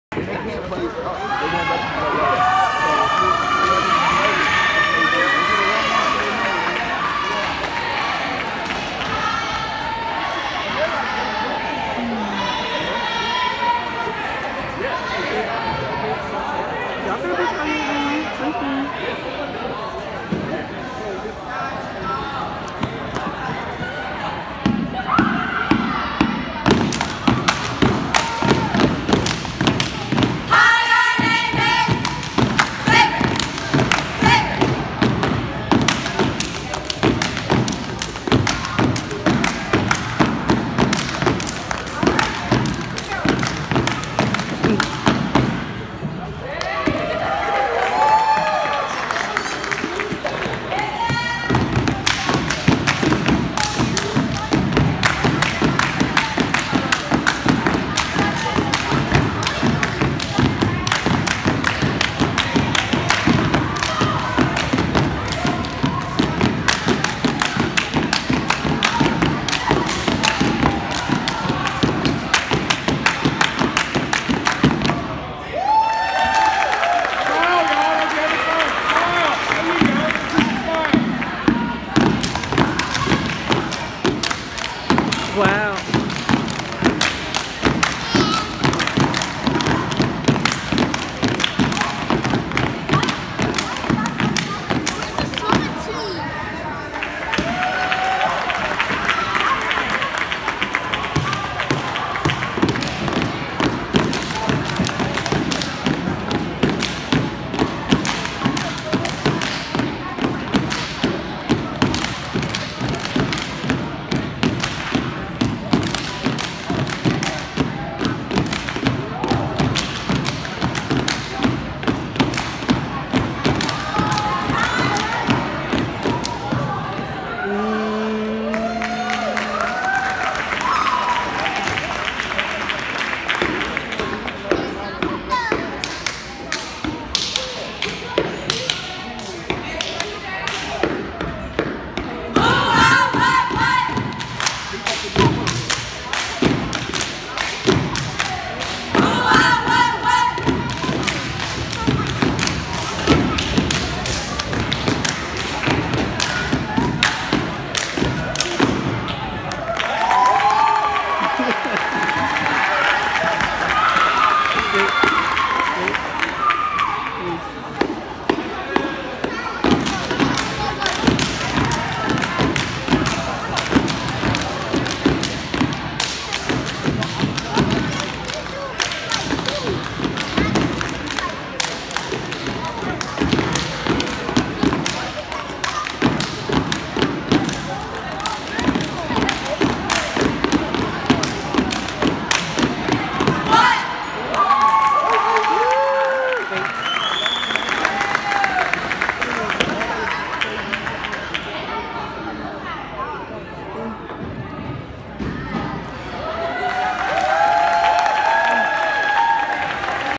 Stonewall Middle School Step Team!
Stonewall Middle School Step Team in Manassas, VA!
Tuesday, February 13, 2018.
Boys’ Basketball Season vs. Metz Middle School.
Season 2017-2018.